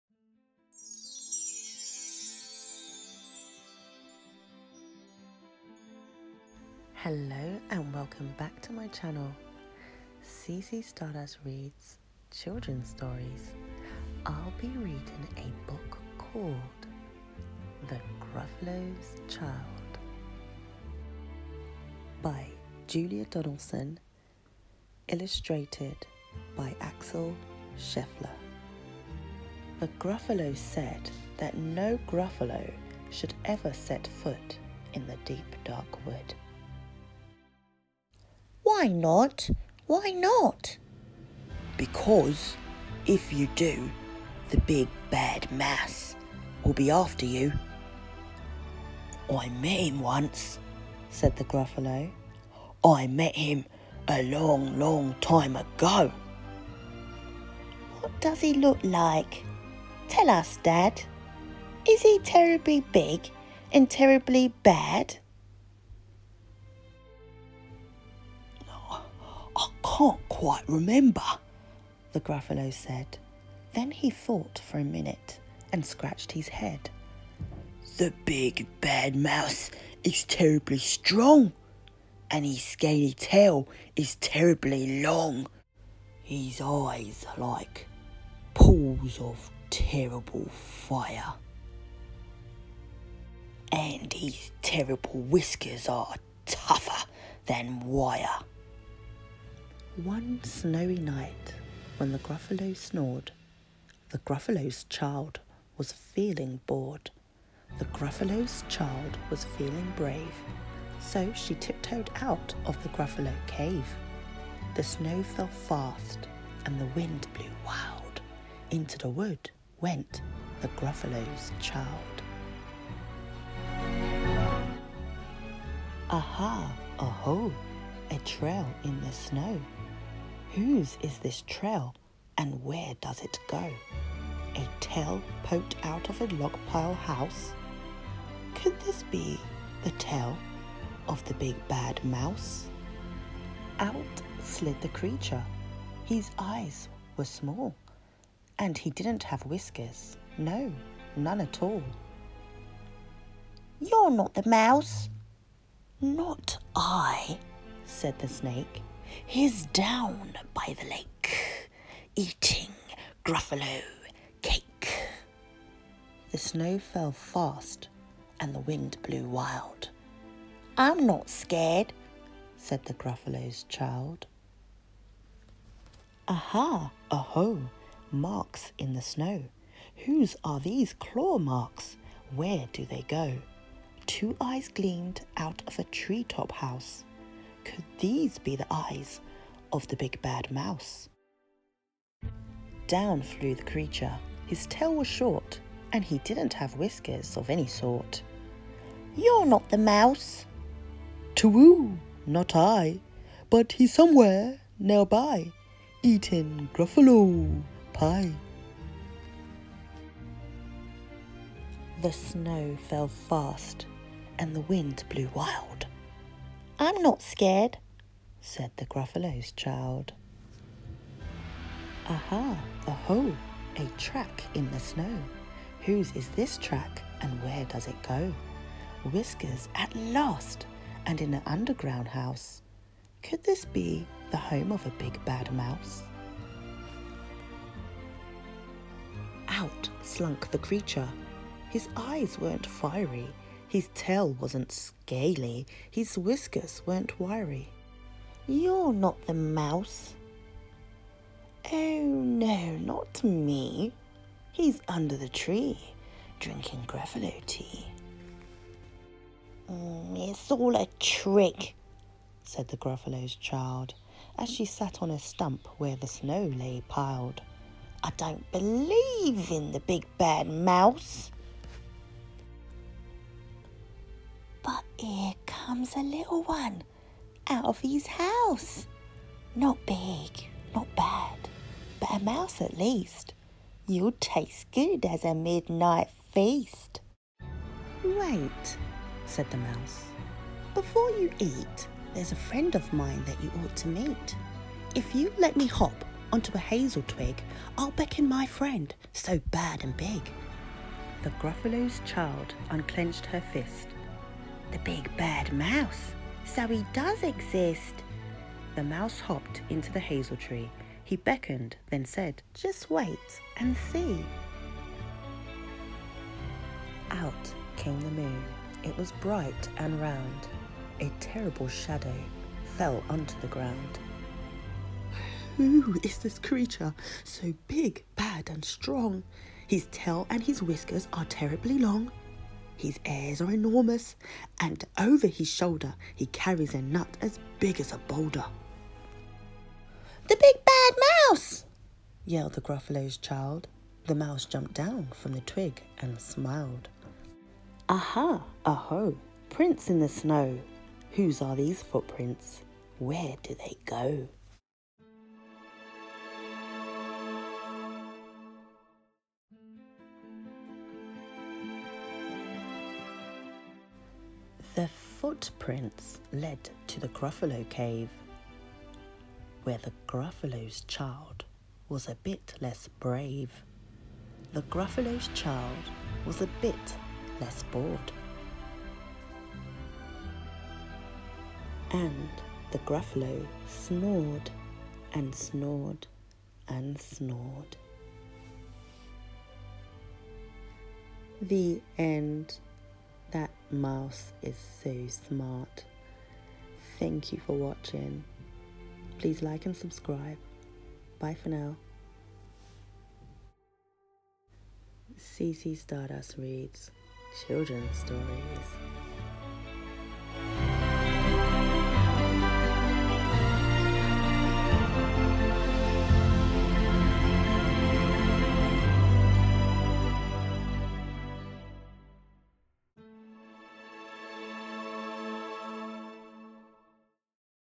Story book reading “The Gruffalo’s Child” read by CC stardust
“The Gruffalo said no gruffalo should
Ever set foot in the deep dark wood.

But one wild and windy night the Gruffalo’s Child ignores her father’s warning and tiptoes out into the snow. After all, the Big Bad Mouse doesn’t really exist….does he?”

By Julia Donaldson
And Illustrated by
Axel Scheffler